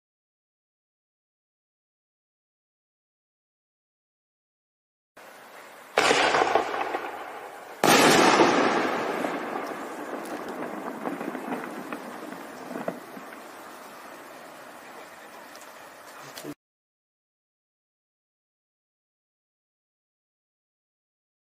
Shell - 2'' Nishiki - PSS062 ... Available at American Wholesale Fireworks!
Shell - 2'' Nishiki (WPL50SS-03-NK) (PSS062) is a 2", 1.4 Pro Shell made by Wizard Pro Line Fireworks!